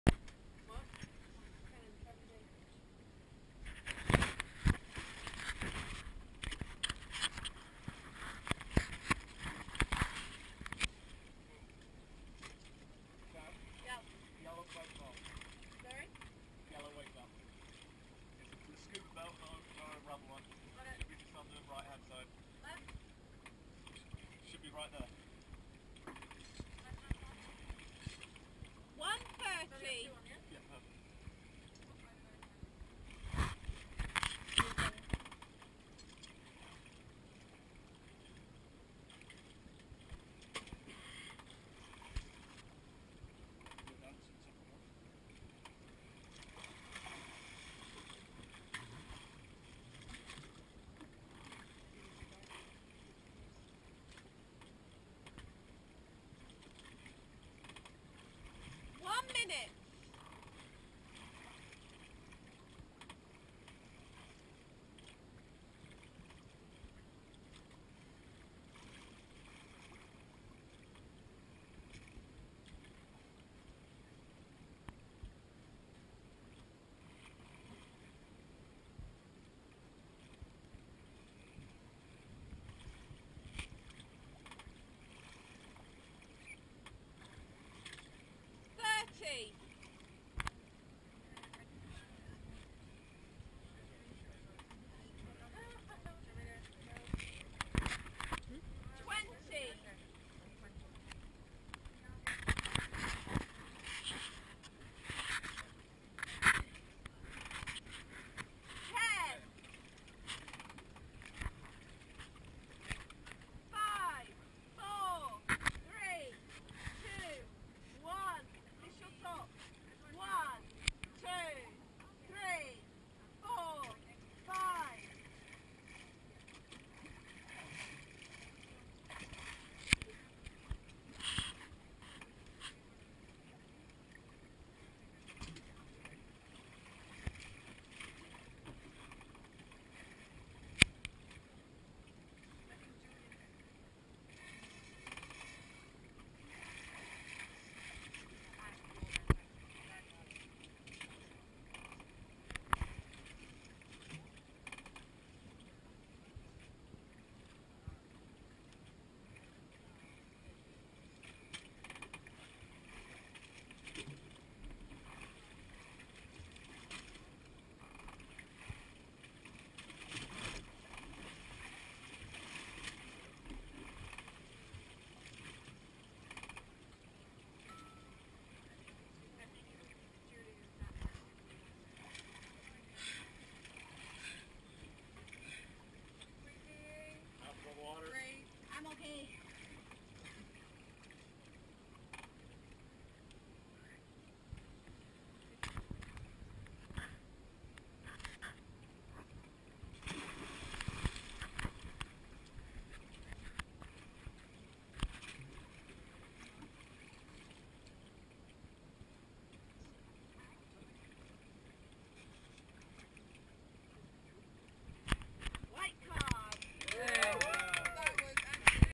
Vivian Chan (Malaysia) Andaman Freediving Competition 2012 - Constant Weight No Fins
Vivian Chan Completes 12m in Constant Weight No Fins discipline during the Freediving Competition this year